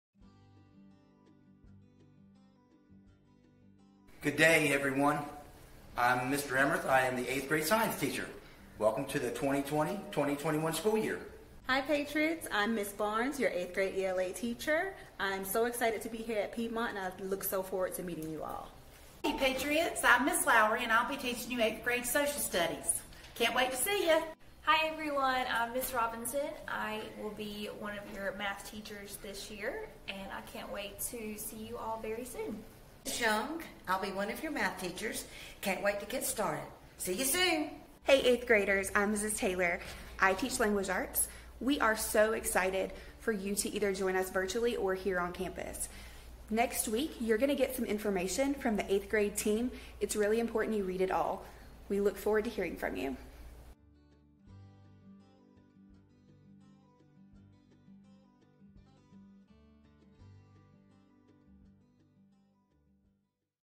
Welcome to 8th Grade
Hey Patriots!  We want to introduce ourselves and welcome you to 8th grade.  We are so glad you are here.